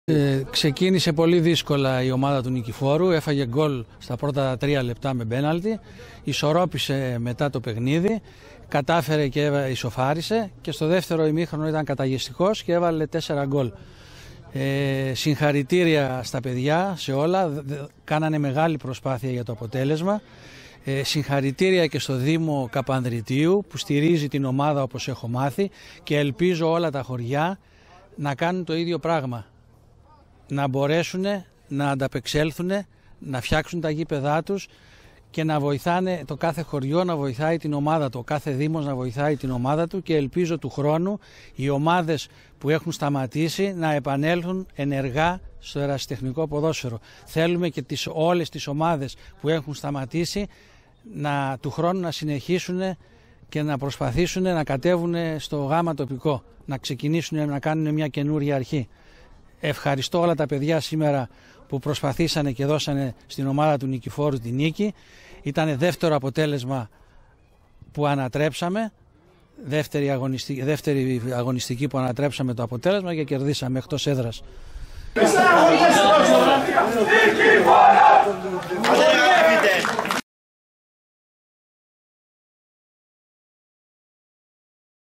Γ ΕΠΣΑΝΑ 22/23: Καπανδρίτι- Νικηφόρος 2-4 |δηλ. Γκαράνης Γιάννης (πρόεδρος ΑΠΟ Νικηφόρος)
Γ ΕΠΣΑΝΑ 22/23
Καπανδρίτι - Νικηφόρος 2-4

δηλ. Γκαράνης Γιάννης
(πρόεδρος ΑΠΟ Νικηφόρου)

📷   Γιώργος Τσιφλίκας   📹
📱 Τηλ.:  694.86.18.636